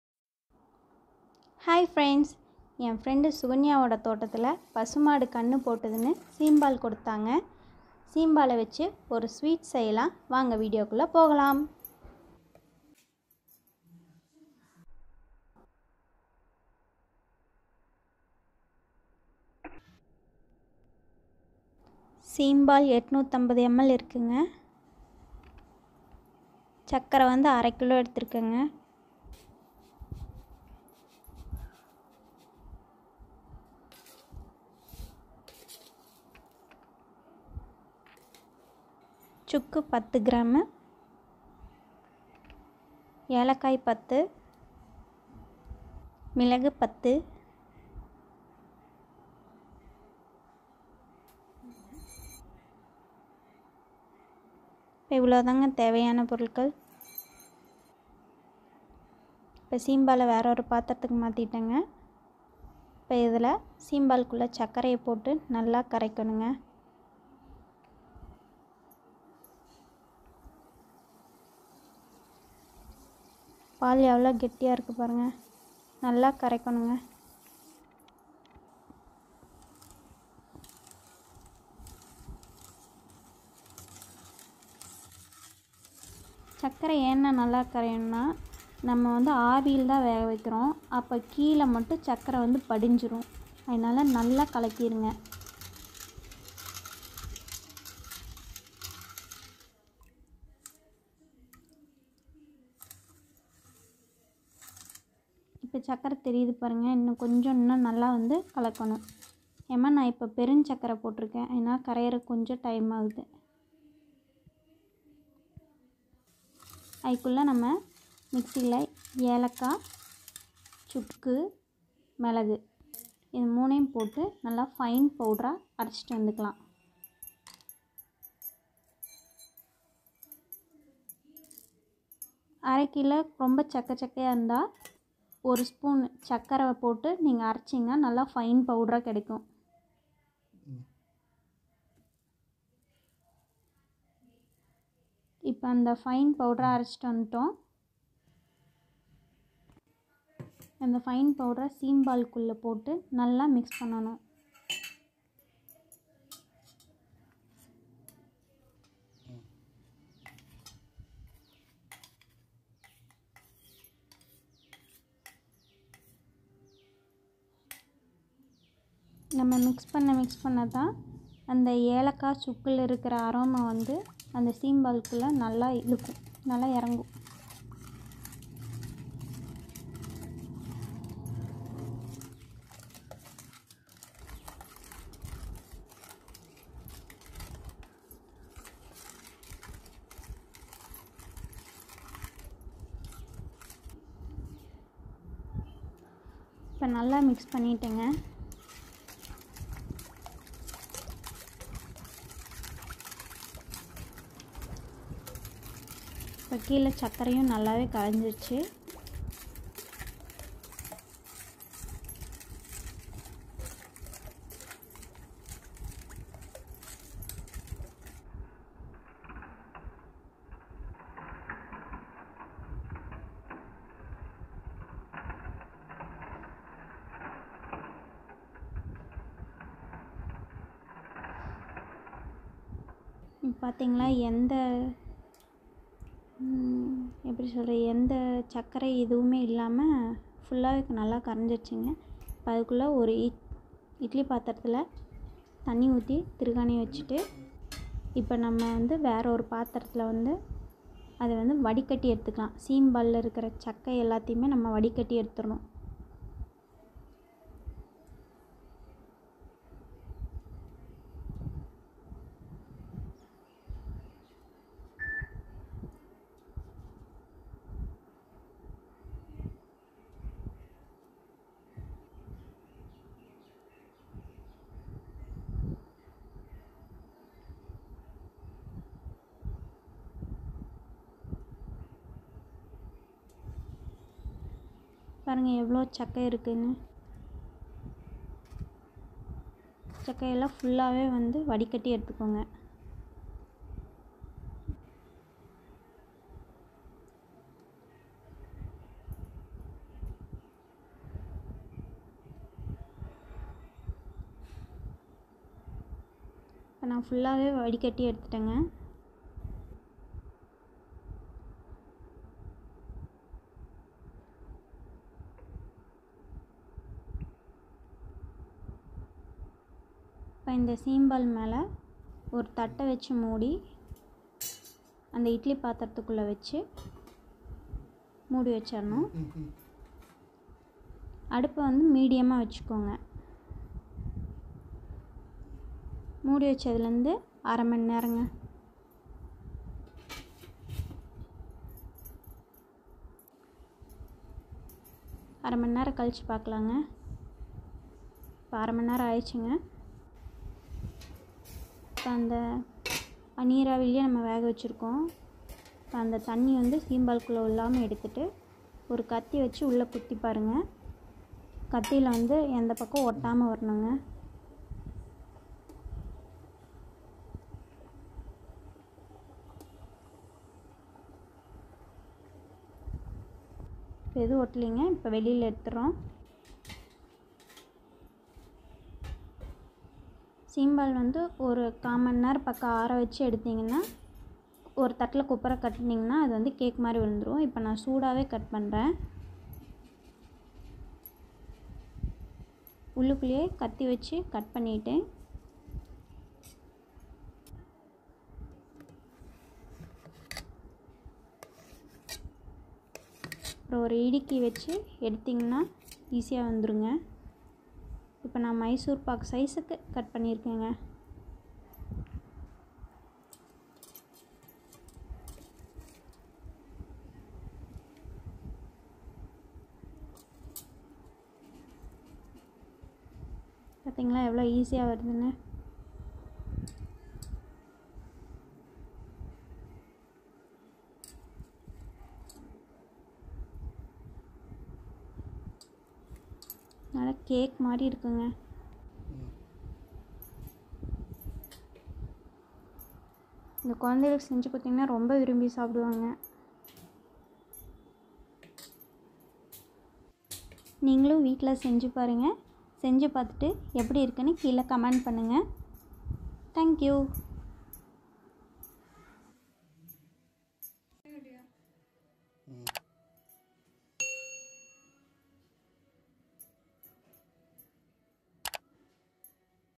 seempal recipe in Tamil / சீம்பால் செய்வது எப்படி / Cow colostrum milk cake / Milk pudding 🍮
சீம்பால் - 850 மில்லி
சர்க்கரை - 500 கிராம்
சுக்கு - 10 கிராம்
ஏலக்காய் - 10 எண்ணிக்கை
மிளகு - 8 எண்ணிக்கை